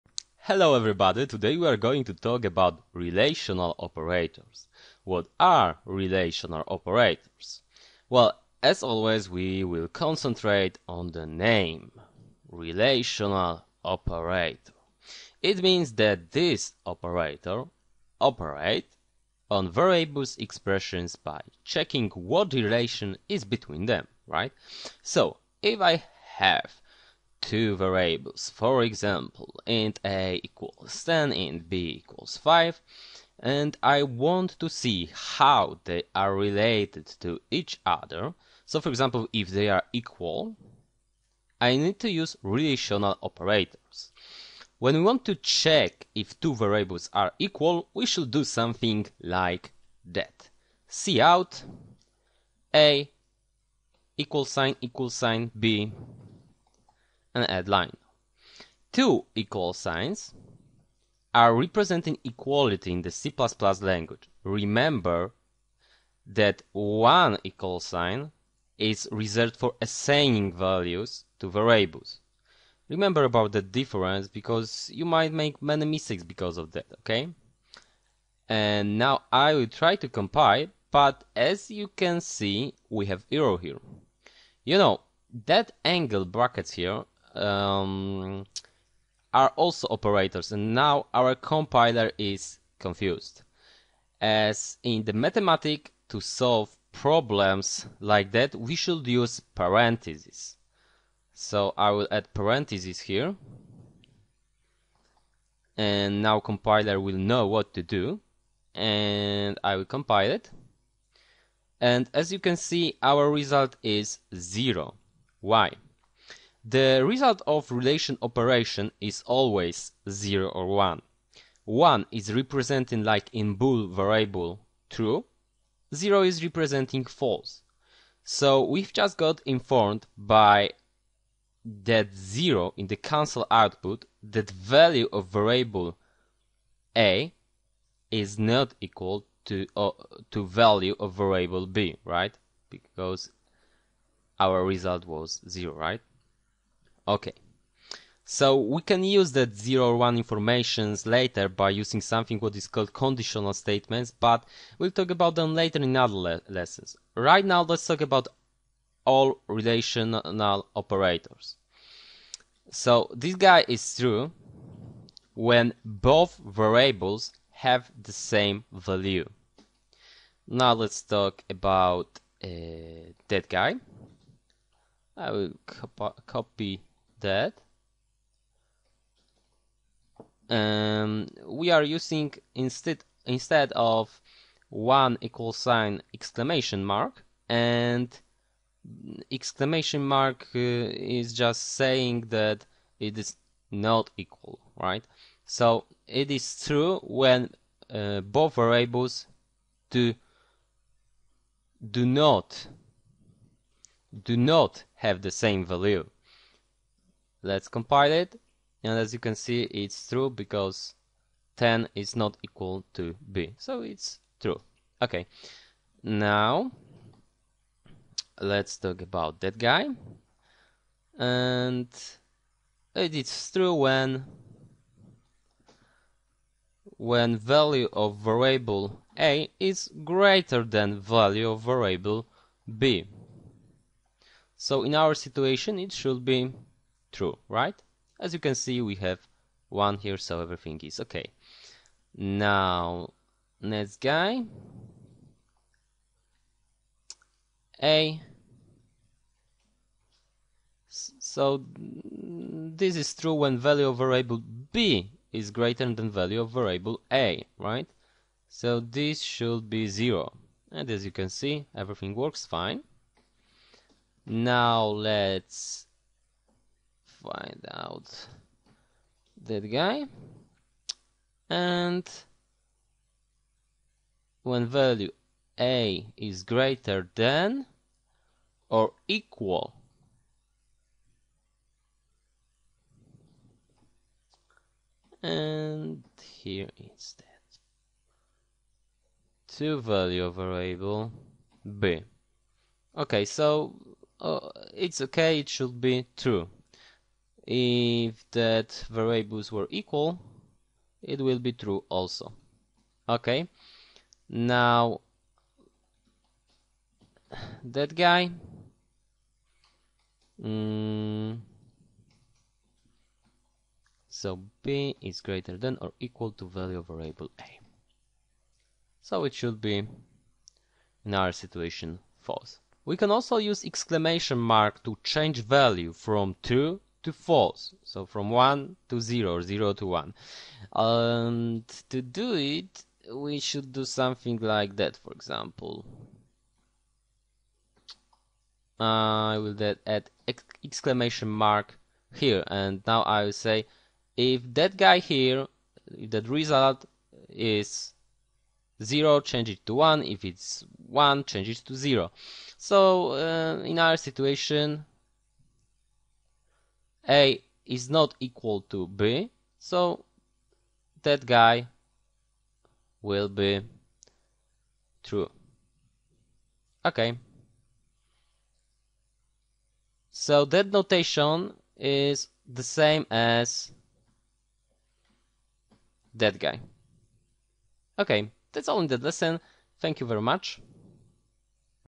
C++ relational operators
We learn how to use and what are relational operators.